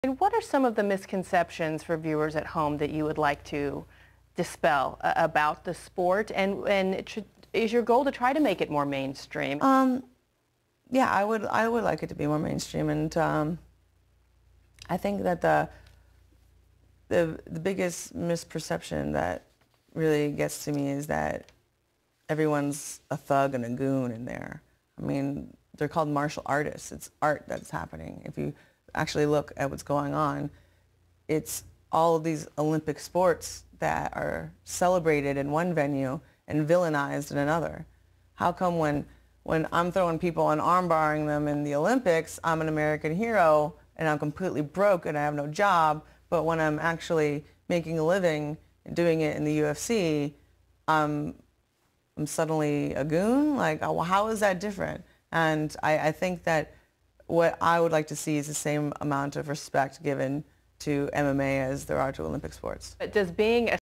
Ronda Rousey on the hypocrisy of social justice warriors
Ronda asks why she is considered a hero when she throws people around and breaks arms at the Olympics while struggling to make ends meet, yet the minute she begins making money for doing the same thing she is criticized for being a thug by the moral majority.